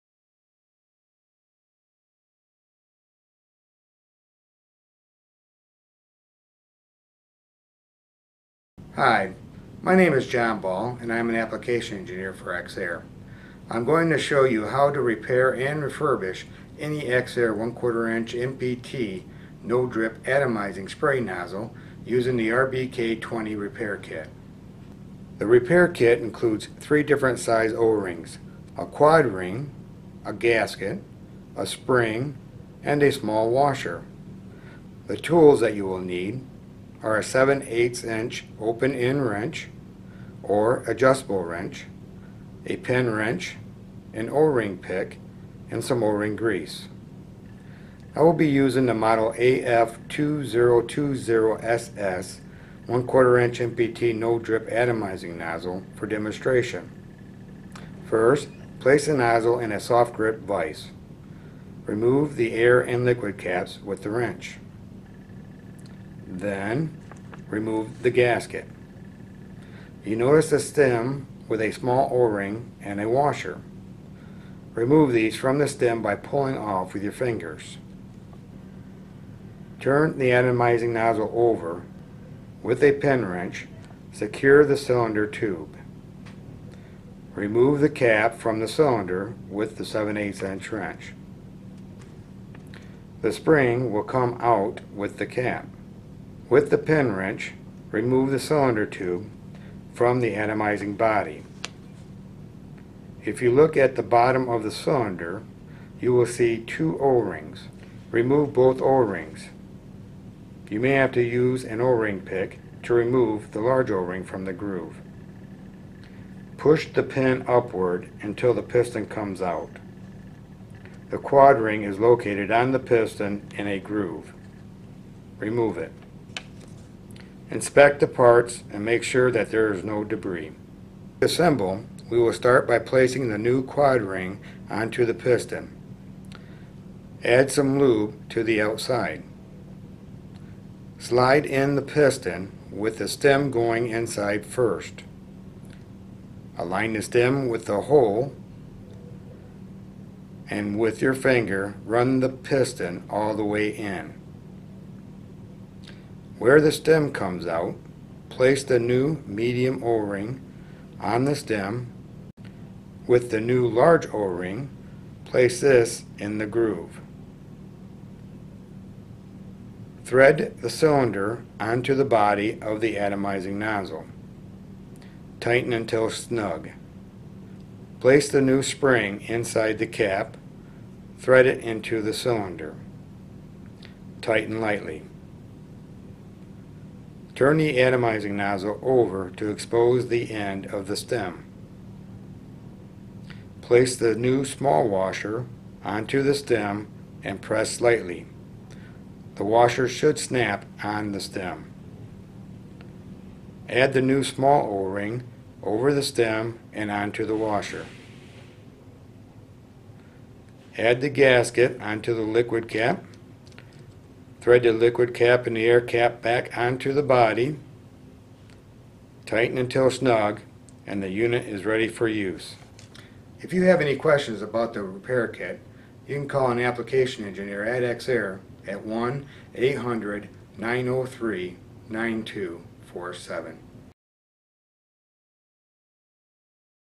How to install the EXAIR No Drip Atomizing Nozzle Repair Kit
This video showcases the parts included as well as how to install the EXAIR No Drip Atomizing Nozzle Repair Kit.  There are three separate models available each corresponding to the NPT size of the atomizing spray nozzle.  RBK90 - 1/8" NPT, RBK20 - 1/4" NPT, and RBK60 - 1/2" NPT.  If you have any other questions, please reach out to an Application Engineer at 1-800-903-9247